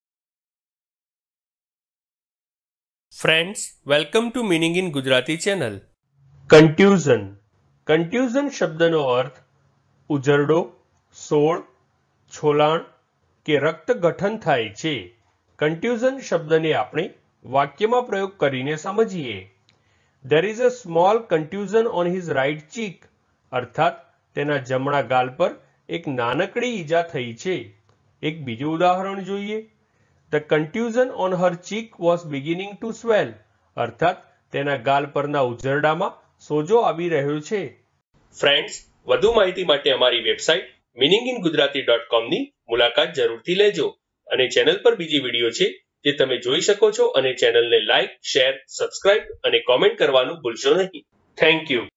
Contusion Meaning in Gujarati | Contusion નો અર્થ શું છે | Contusion in Gujarati Dictionary
આ વિડિયો માં તમે Contusion નો Gujarati માં અર્થ સમજશો અને એની સાથે Contusion નું pronunciation પણ શિખશો. એટ્લે કે તમે Contusion ના meaning ની સાથે સાથે એ પણ શિખશો કે Contusion ને કેવી રીતે બોલાય, Contusion ને બોલવાની સાચી રીત કઈ છે.

તો બસ એક મિનટ માં શીખો Contusion શબ્દ ને.

Contusion ને examples થી સમજીએ